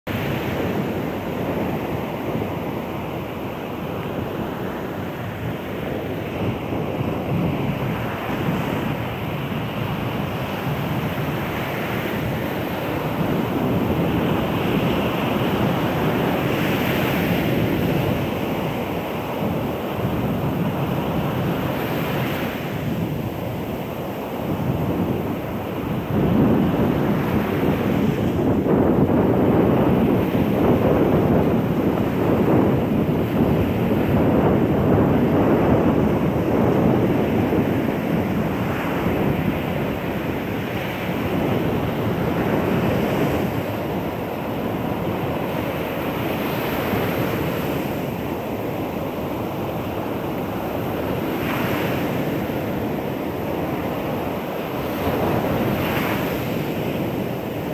20201127 море штормить
бушует море, Одесса